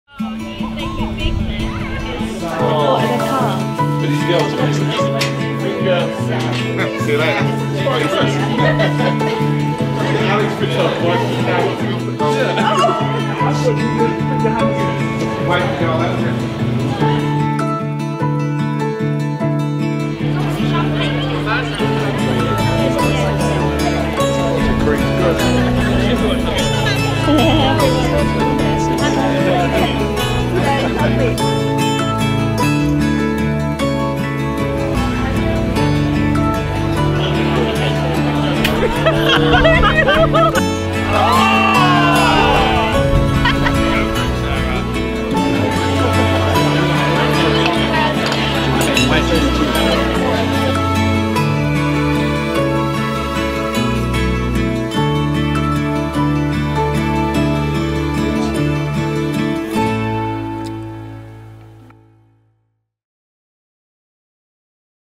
DIY Farm Wedding and Cowshed
Farm wedding in the cowshed with flowers in milk bottles